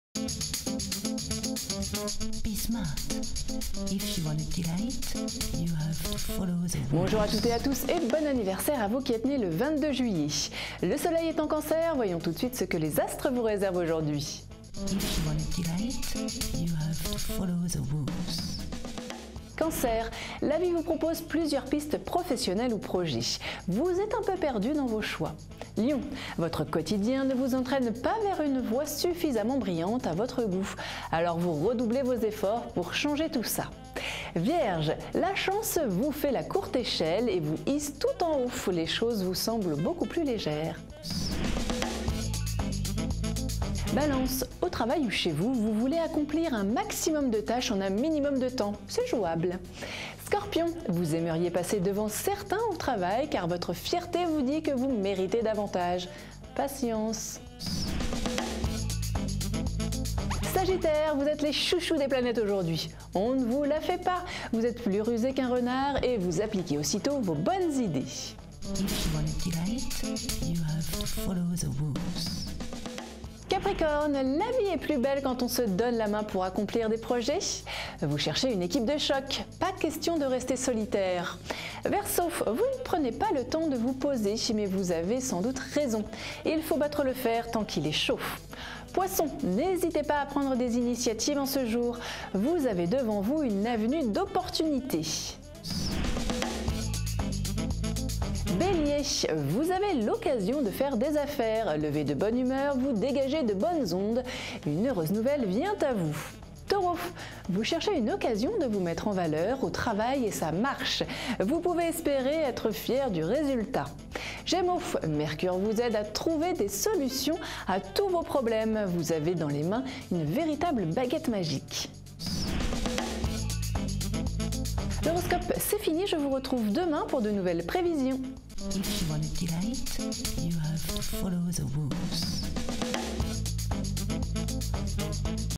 Horoscope du jour gratuit du 22 Juillet 2015 par Astrocenter
Que vous réserve ce 22 Juillet 2015 en amour, en famille et au travail ? Dans son horoscope du jour Ema Fontayne vous dit tout ! Quel que soit votre signe astrologique pour bien commencer votre journée retrouvez votre horoscope quotidien gratuit en vidéo.